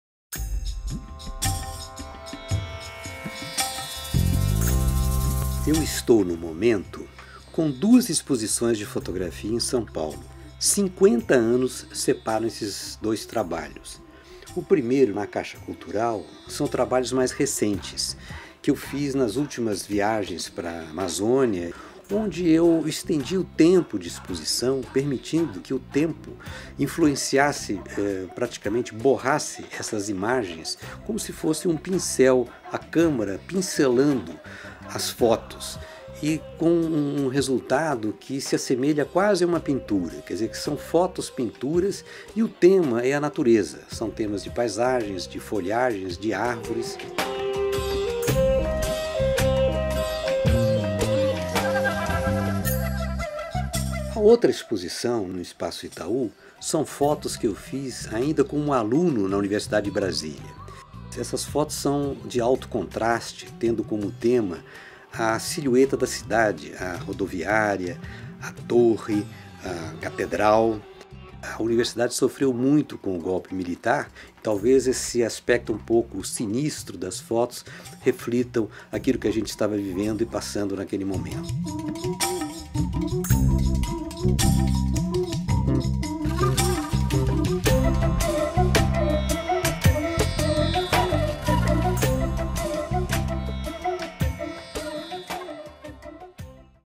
Exposições fotográficas de Jorge Bodanzky em São Paulo
Cinquenta anos separam duas séries fotográficas do artista e cineasta Jorge Bodanzky, em exposições distintas em São Paulo.
"In Natura", na CAIXA Cultural São Paulo traz suas imagens mais recentes, até 14 de maio de 2017.
"Brasília", suas imagens mais antigas quando ainda era aluno da UnB, em exposição no Espaço Itaú de Cinema até 02 de julho de 2017.